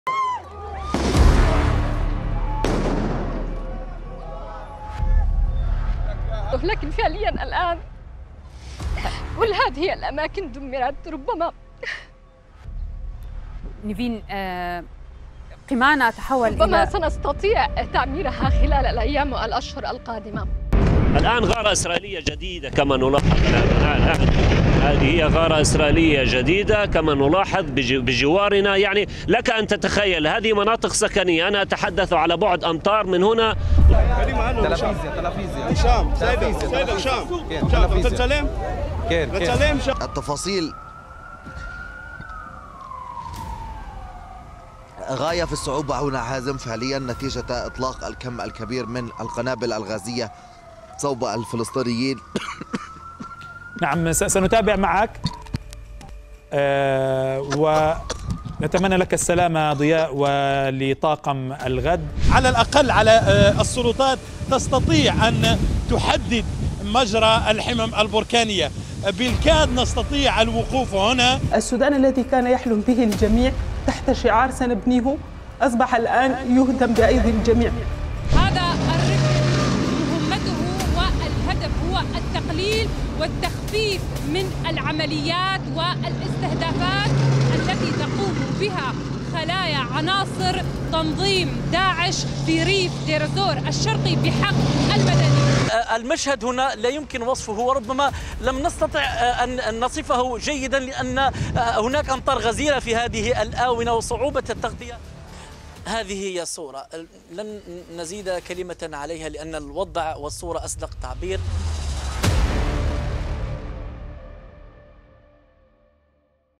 لا مكان آمن.. لحظات رعب رآها مراسلو الغد في 2021
من تحت القصف في غزة الصامدة ومن أقرب نقطة للبركان في إسبانيا وفي قلب الاشتباكات رفقة الخطر خلال 2021.. مراسلو الغد كانوا في كل حدث ينقلون لكم